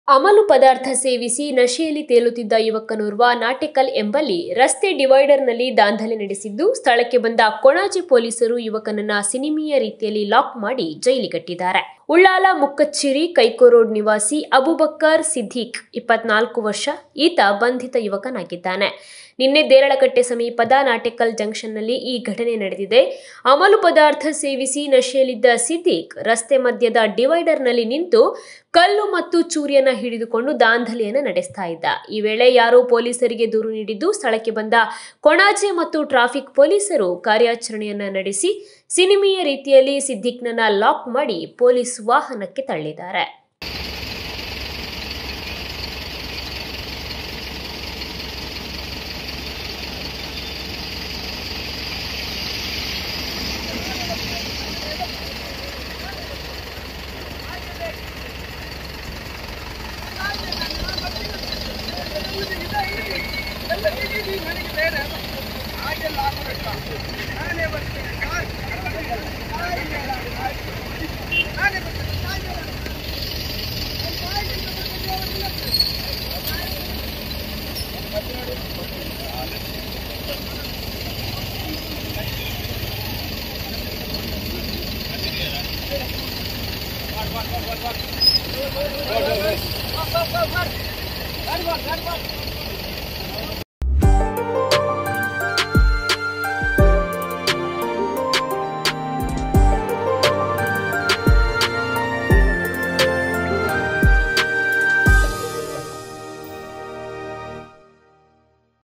ನಾಟೆಕಲ್ ನಲ್ಲಿ ಅಮಲಲ್ಲಿ ತೇಲುತ್ತಿದ್ದ ಯುವಕನನ್ನು ಎತ್ತಿಕೊಂಡು ಬಂದು ಜೀಪ್ ಗೆ ಹಾಕಿದ ಪೊಲೀಸರು | News Karnataka
ನಾಟೆಕಲ್ ನಲ್ಲಿ ಅಮಲಲ್ಲಿ ತೇಲುತ್ತಿದ್ದ ಯುವಕನನ್ನು ಎತ್ತಿಕೊಂಡು ಬಂದು ಜೀಪ್ ಗೆ ಹಾಕಿದ ಪೊಲೀಸರು